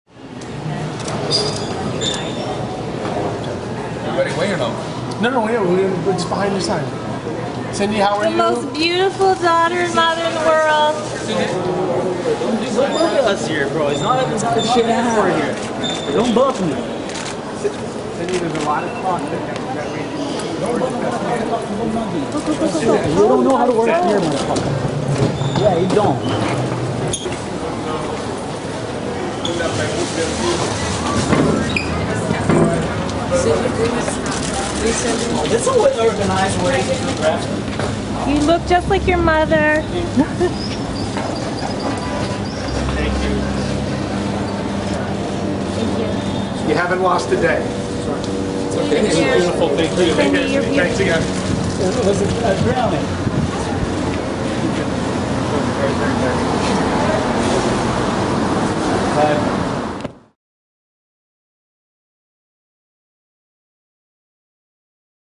Crawford and look-a-like daughter arrive...
The 13-year-old is fast becoming the spitting image of her supermodel mom, from her fashion choices right the way down to her stance.